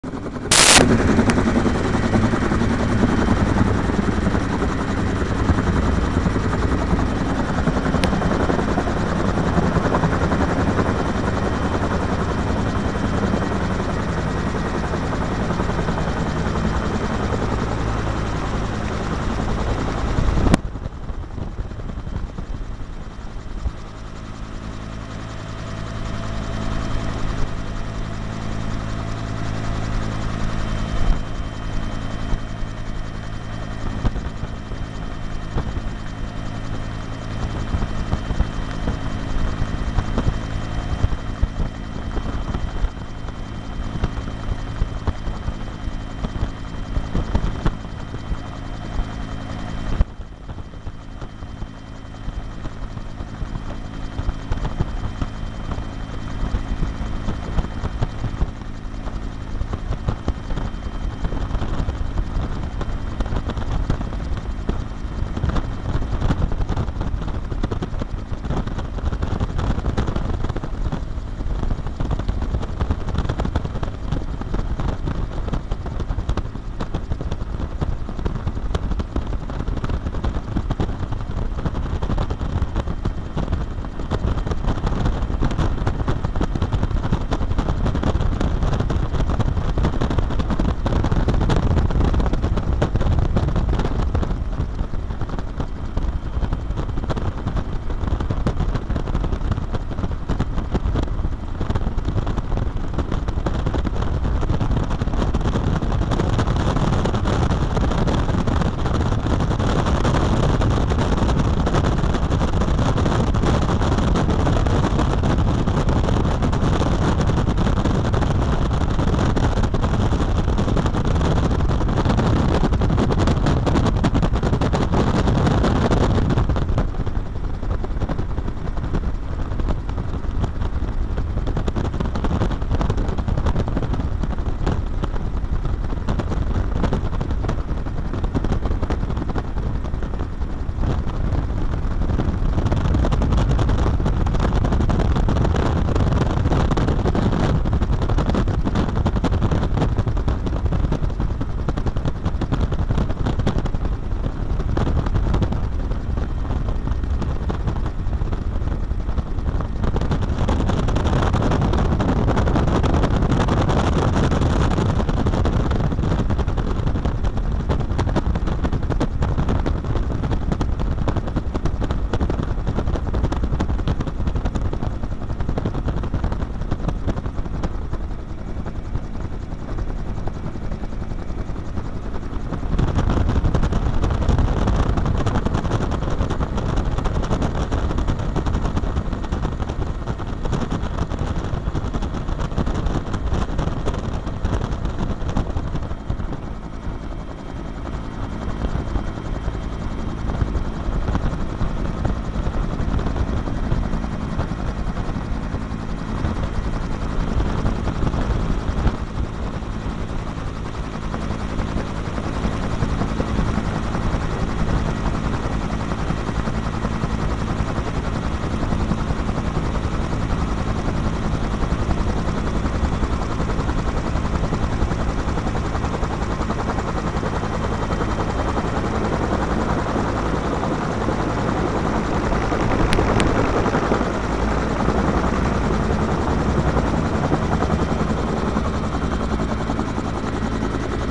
Helicopter Ride To The JPSO SWAT Training Facility
The Second Part Of My Over The Edge Experience. Helicopter Ride In A UH-1 With The Doors Open.